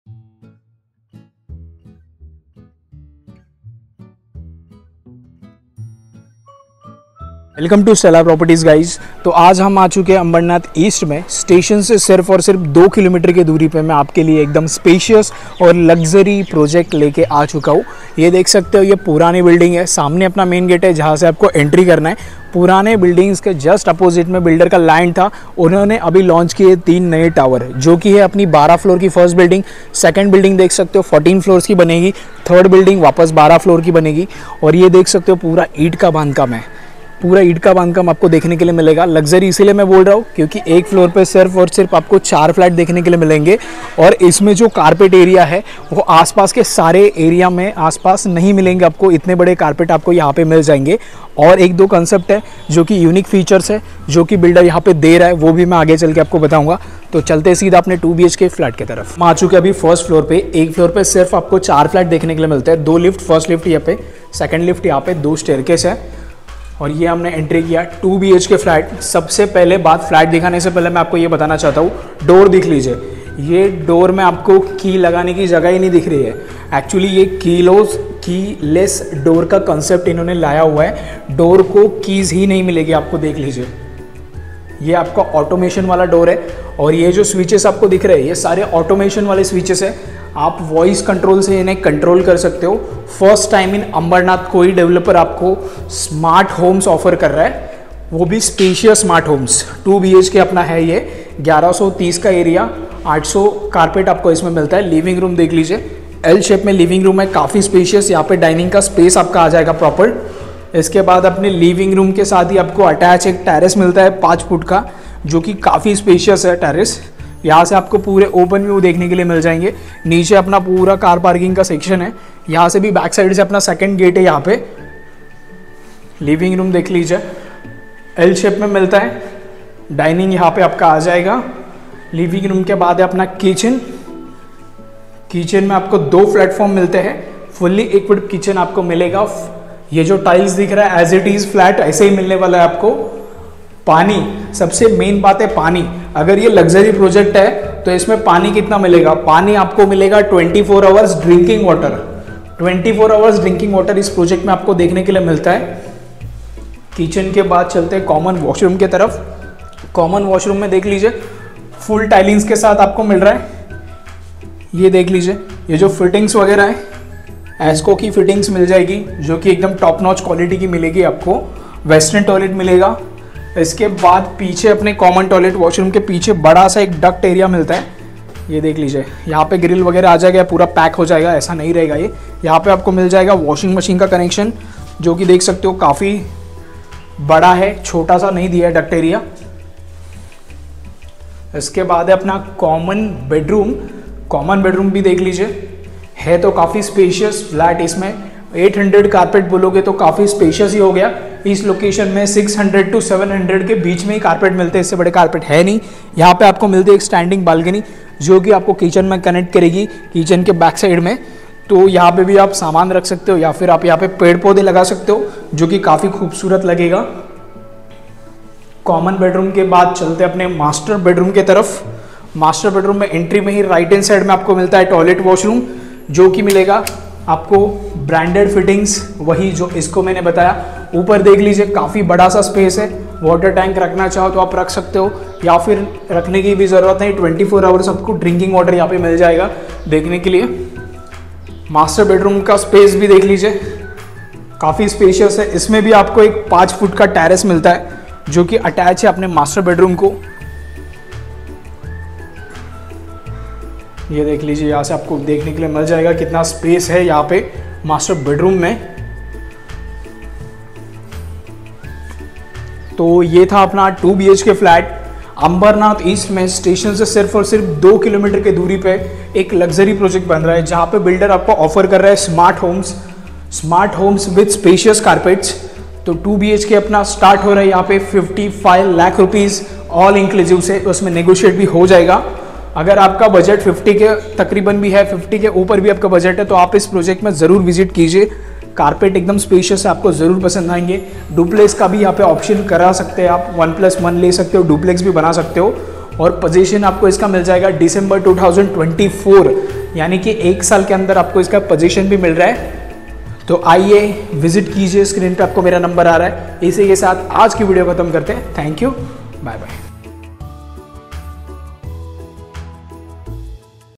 2BHK SMART HOMES | 800 Sq.Ft + Carpet Area | Ambernath स्टेशन पासुन फक्त 8-9 मिनिटे अंतरावर..
2BHK SMART HOMES | 800 Sq.Ft + Carpet Area | Ambernath स्टेशन पासुन फक्त 8-9 मिनिटे अंतरावर..

Location -Ambernath East
Just 2Km From Ambernath Station
8-9 Minit Drive Distance from Station..

Still+12 & Still+14 Floor Building's
10 Building Complex
Possession December -2024

2Bhk Starts From 800 Sq.Ft Carpet
Price Starts From 55 Lakh All inclusive ( Negotiable )
First Time In Ambernath ( Full Automation Homes)
Keyless Door In All Flats
All Basic Amenities Available
Hurry up..